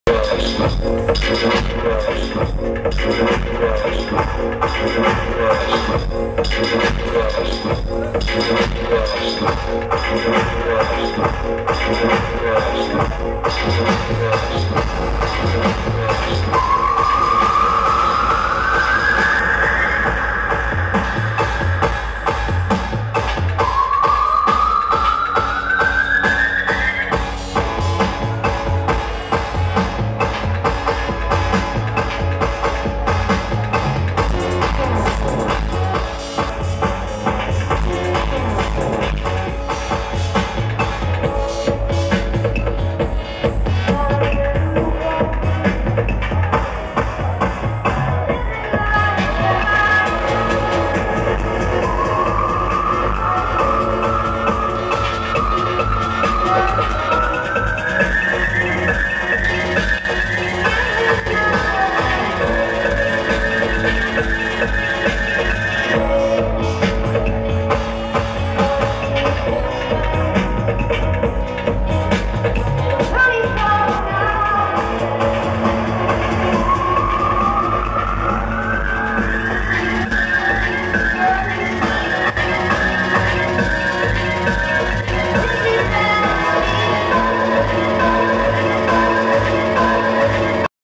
Daedelus Movement Festival 2011, Detroit MI
Daedelus performing at the Movement Festival (DEMF) 2011, Detroit MI

Footage from OFtv contributors.